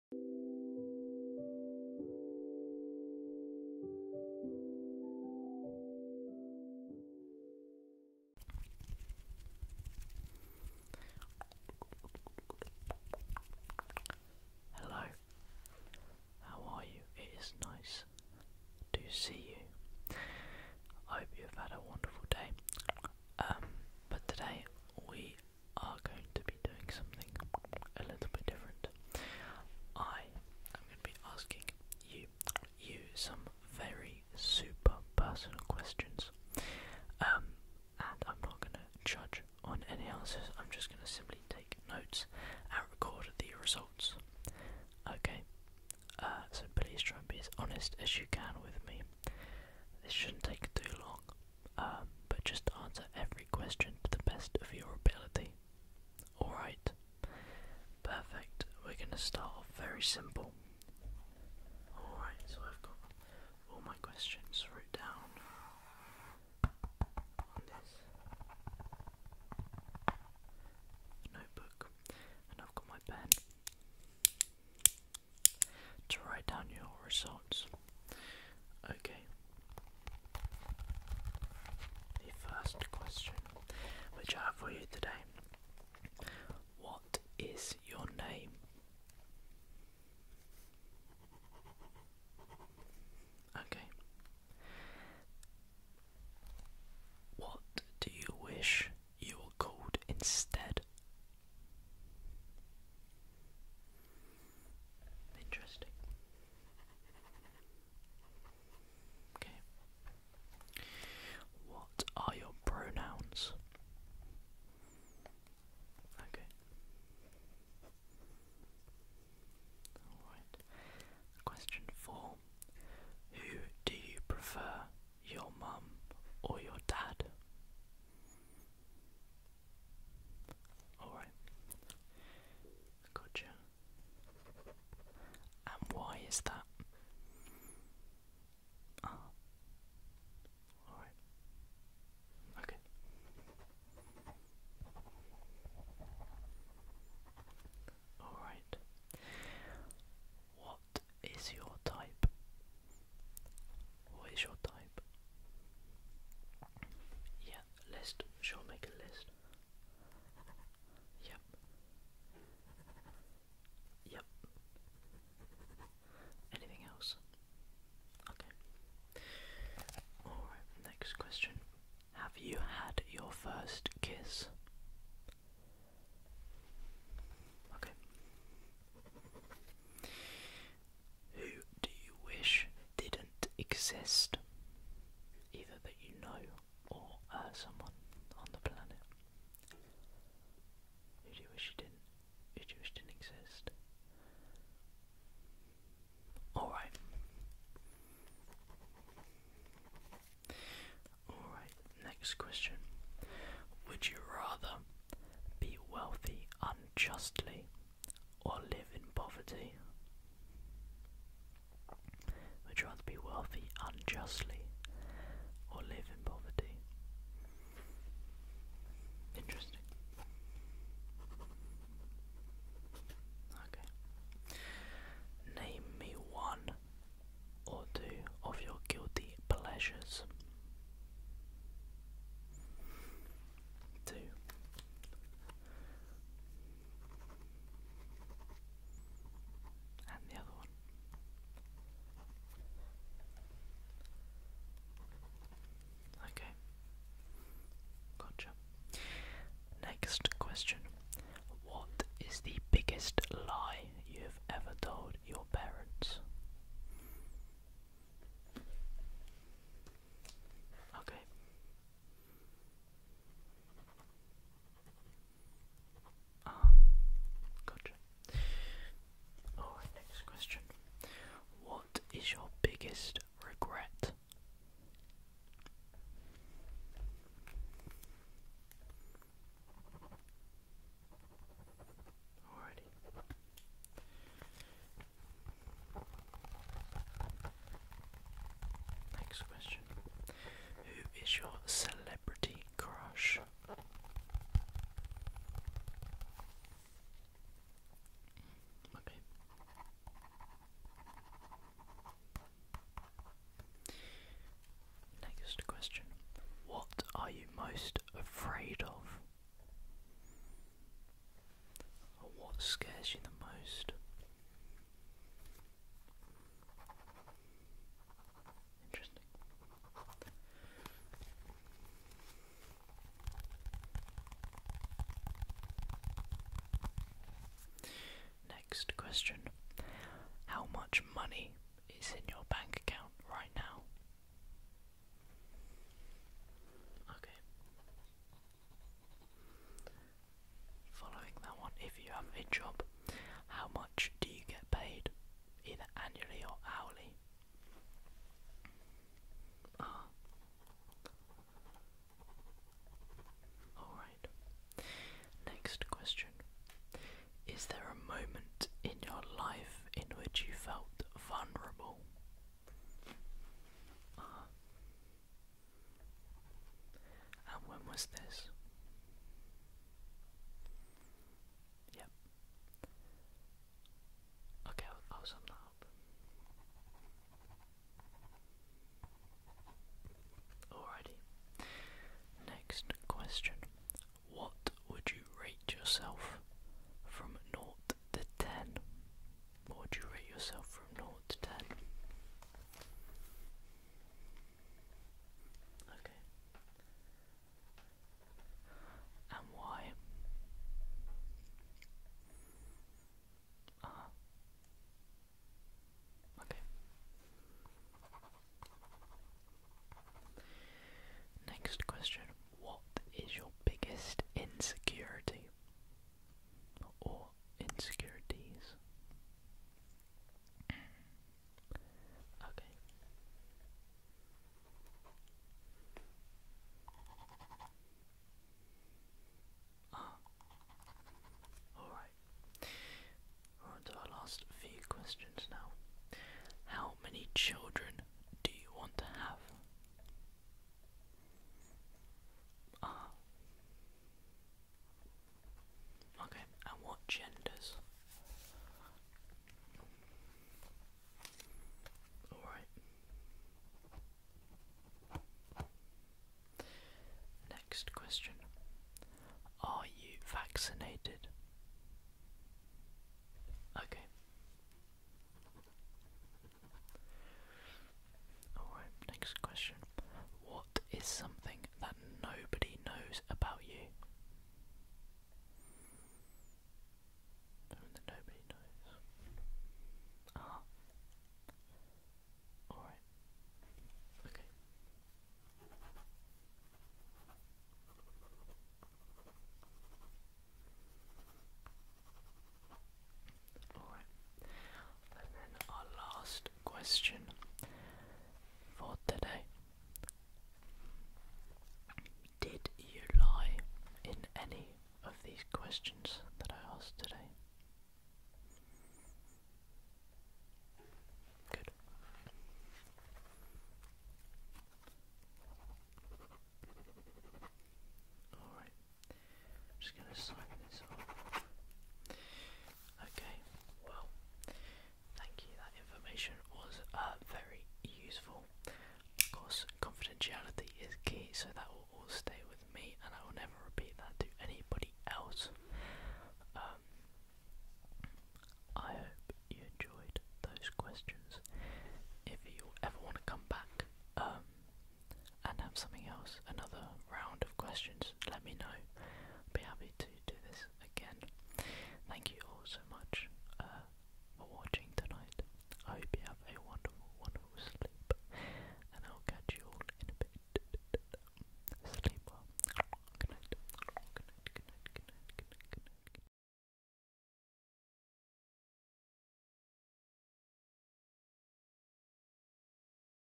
Asking You Super Personal Questions | ASMR
Hi guys!

Welcome back to another video! In today's video I will be asking you a variety of super personal questions!

I hope you enjoy this video! If you do, please be sure to let me know by leaving a like!

Peace